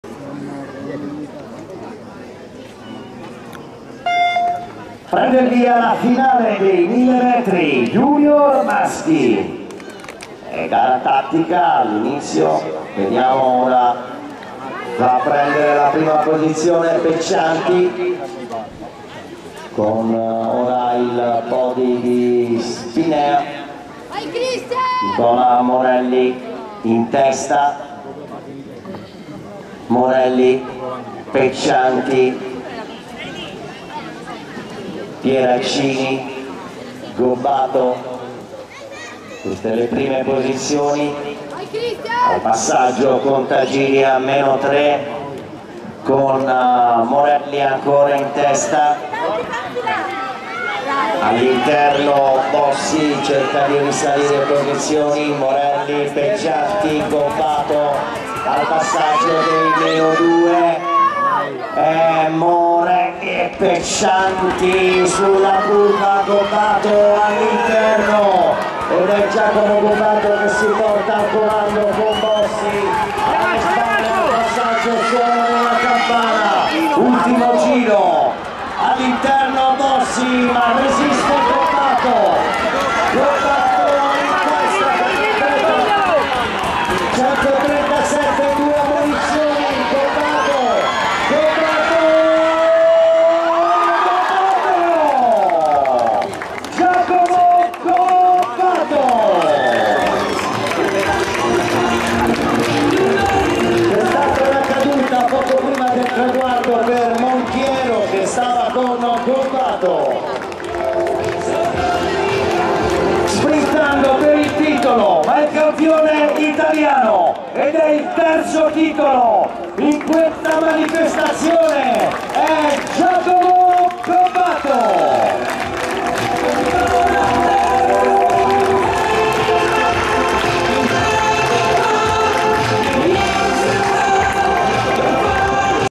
Campionati Italiani Pista A-J-S 2024 - J/M 1000m sprint, finali
Campionati Italiani "Pista" Allievi-Juniores-Seniores 2024
7 Luglio 2024, San Giorgio delle Pertiche (PD) - Italy.
Organizzazione: A.S.D Skating Club Pertichese.
Categoria Juniores Maschile.
1000 metri sprint, Semifinali.

Risultati
1 393 GOBBATO GIACOMO 752 PATT.SPINEA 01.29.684
2 537 DI PAOLA ANDREA 1260 ROLLING BOSICA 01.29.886
3 137 MONCHIERO FABIO 2564 SAVONA IN LINE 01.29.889
4 46 BOSSI LEONARDO 310 POL. BELLUSCO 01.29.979
5 551 PIERACCINI MARCO 302 S.S.D.S.MENS SANA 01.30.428
6 261 PECCIANTI ALESSANDRO 88 GRUPPO P.S.MINIATO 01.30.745
7 580 SCASSELLATI CRISTIAN 836 FORTITUDO FABRIANO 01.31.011
8 113 MORELLI NICOLA 752 PATT.SPINEA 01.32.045

Diffida al n. 137 MONCHIERO FABIO